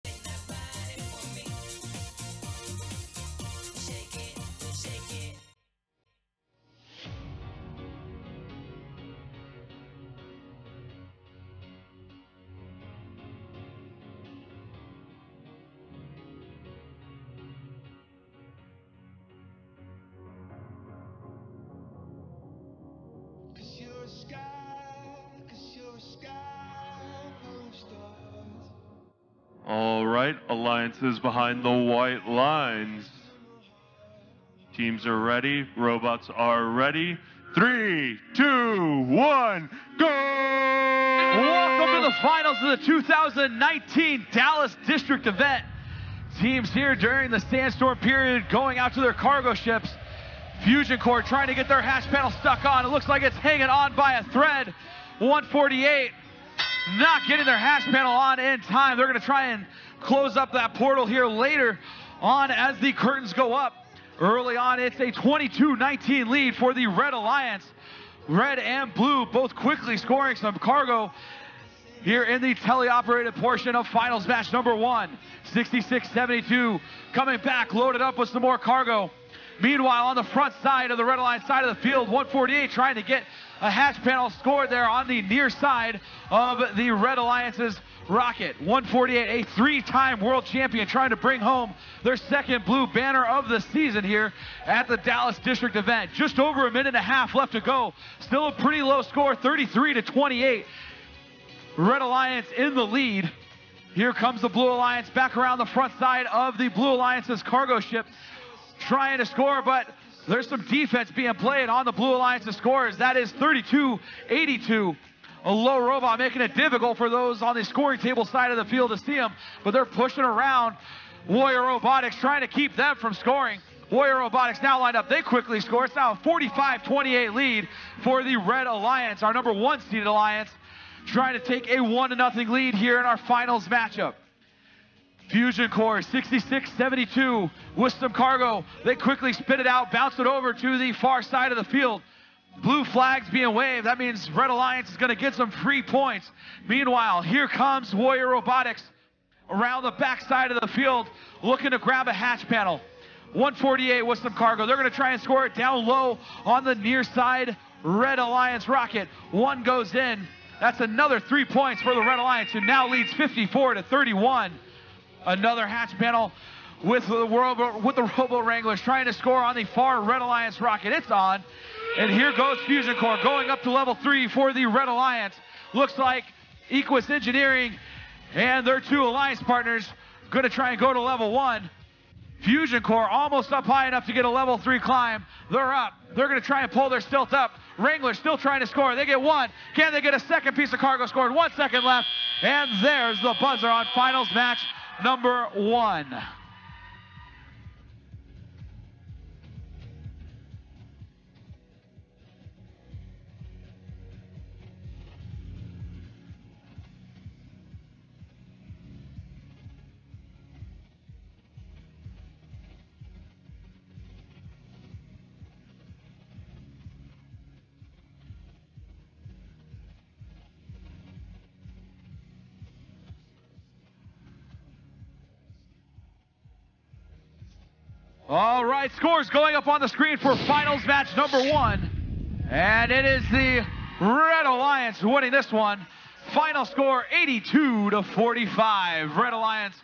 Dallas 2019 F1
Texas FIRST Robotics Competition

Finals 1 2019 FIT District Dallas Event
Teams Score